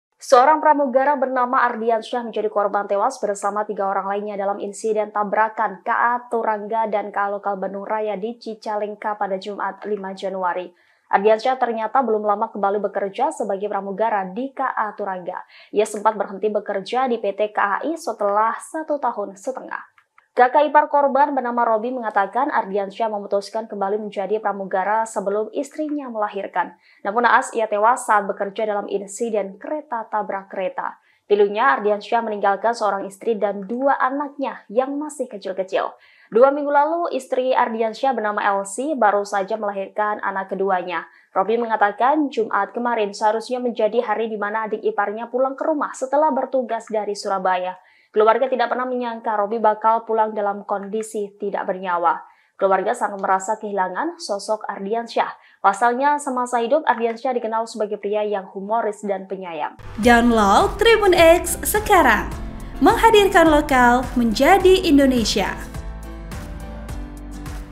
CERITA PILU Pramugara KA Tarungga, Sempat Berhenti Kerja Hingga Istri Lahiran Sebelum Kecelakaan
TRIBUN-MEDAN.Com, Pramugara KA Tarungga, Ardiansyah yang menjadi korban tewas dalam kecelakaan menjadi sorotan.

Ardiansyah baru dikarunia anak kedua sebelum terjadi kecelakaan.

Tak hanya itu, ternyata Ardiansyah sempat berhenti bekerja di PT KAI.

Robby, kakak ipar korban mengatakan Ardiansyah memutuskan kembali bekerja sebagai pramugara sebelum istrinya melahirkan anak kedua.